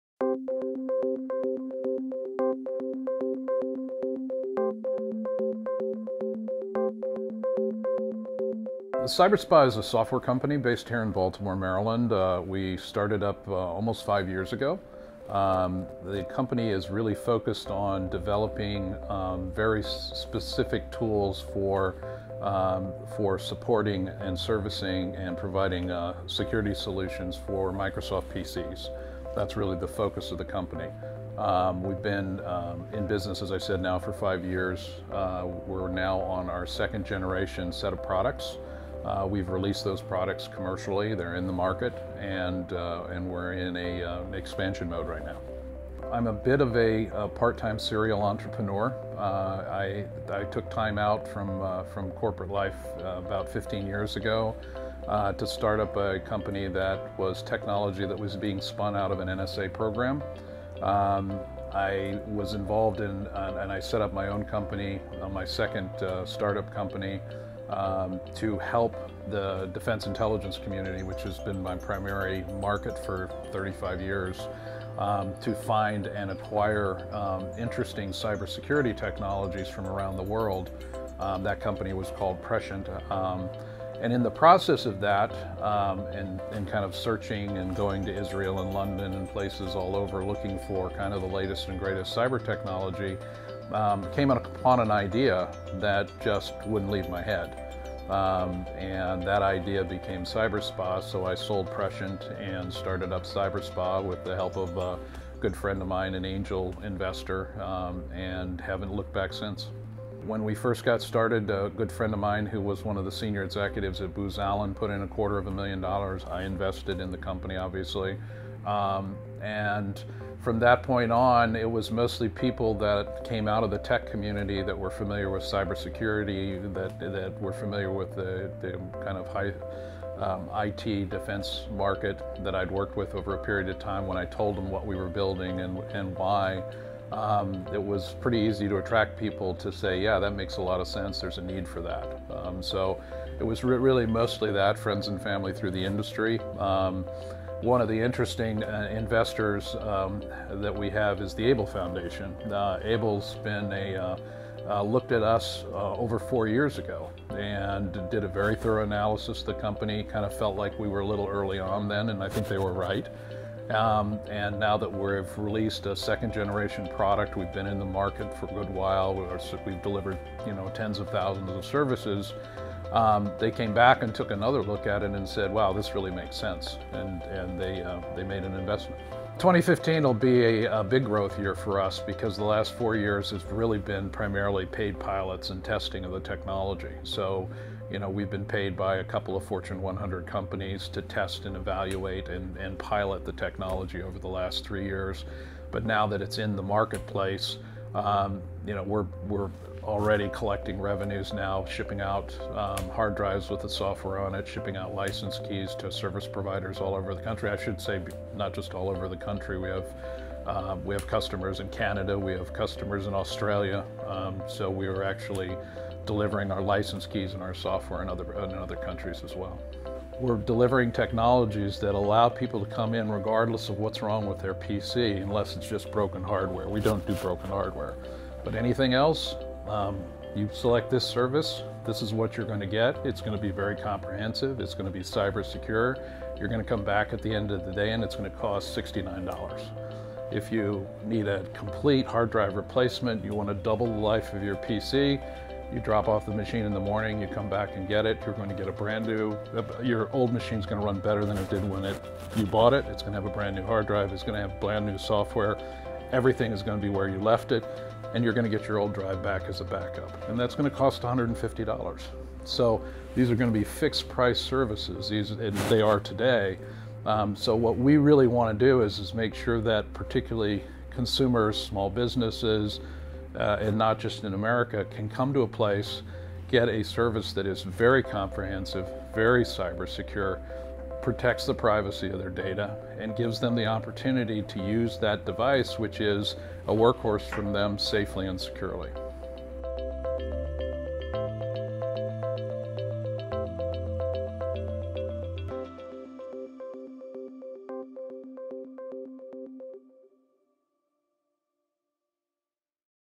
A Conversation with Allen Shay, Co-Founder and President of CyberSpa - Part II
Allen Shay is the co-founder and president of CyberSpa, a Baltimore-based software technology company that develops tools for automating hard drive replacement and offers PC services through its business and charitable partners.